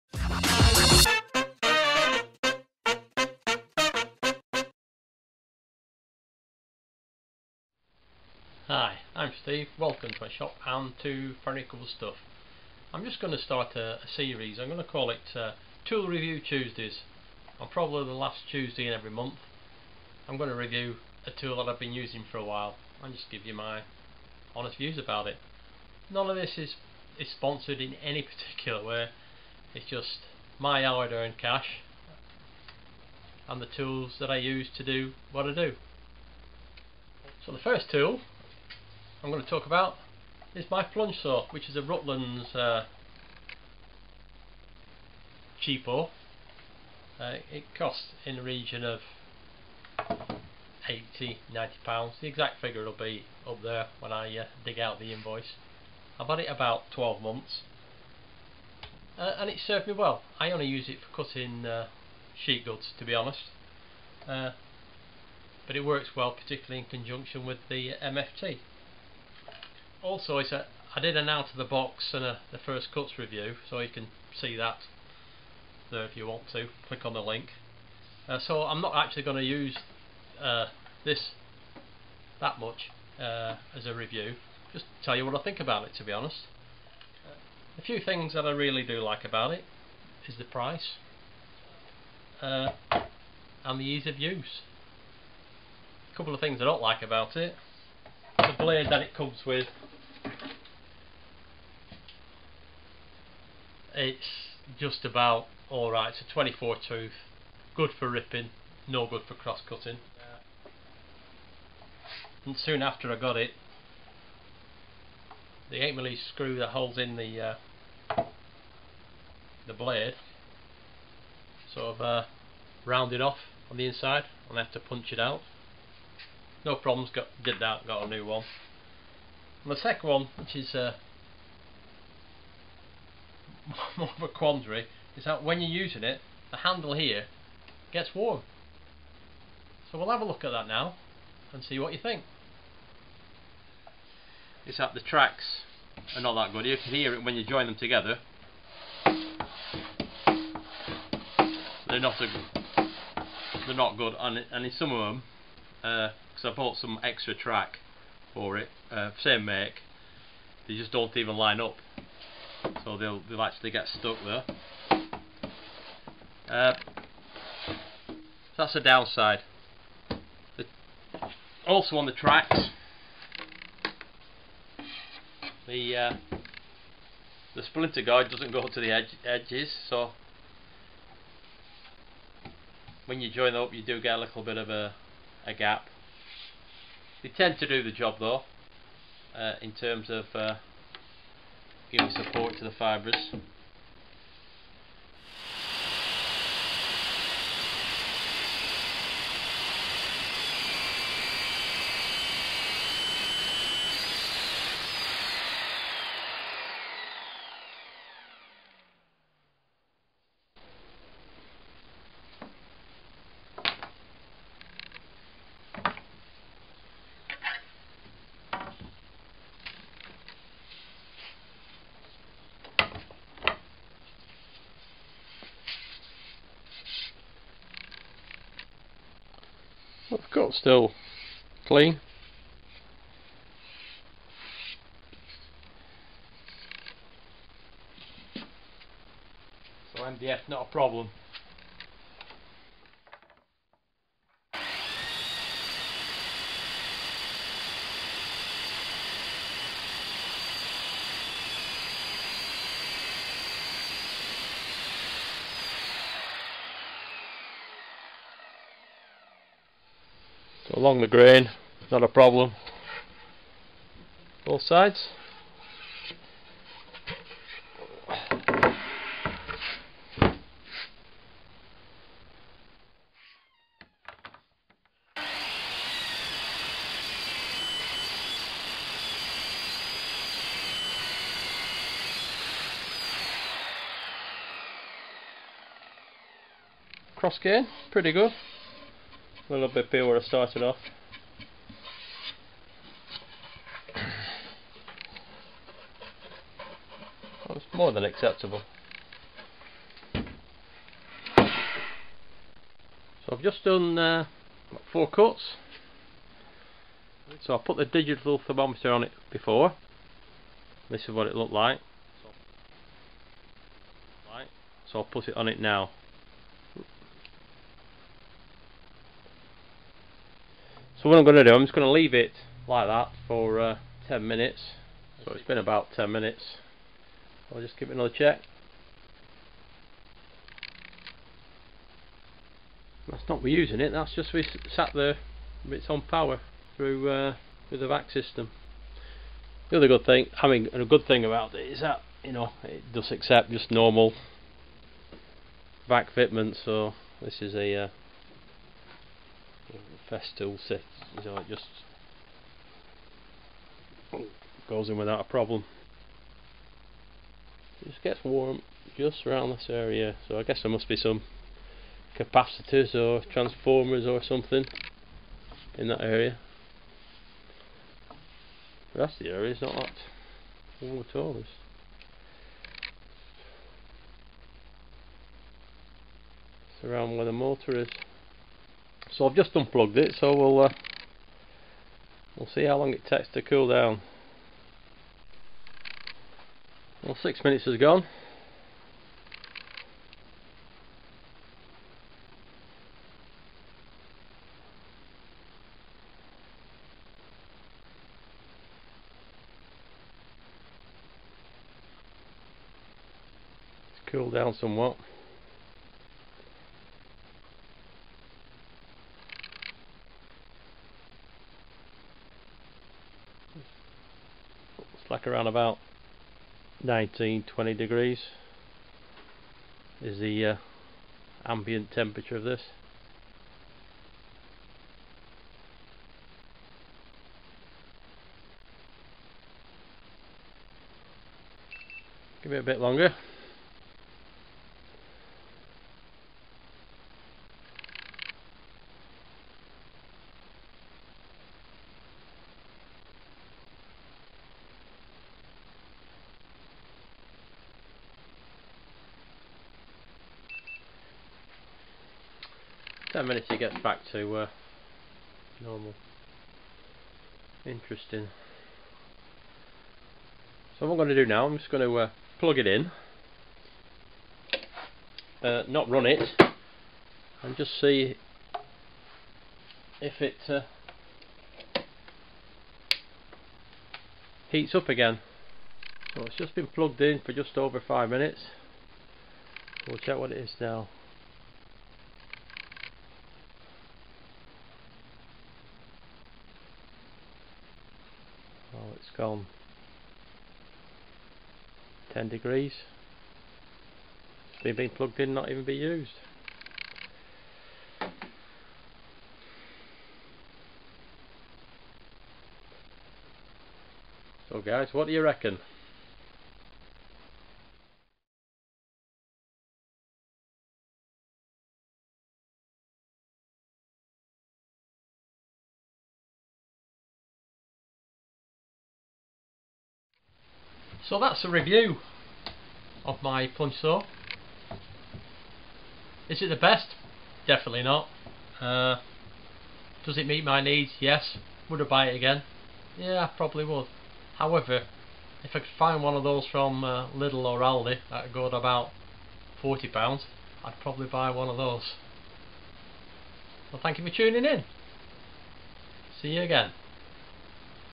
Rutlands Pro Series Plunge Cut Saw XT3516 - Tool Review Tuesday
After approx 18 months of ownership and use I review the +ve's & -ve's of the Rutlands  budget plunge cut saw.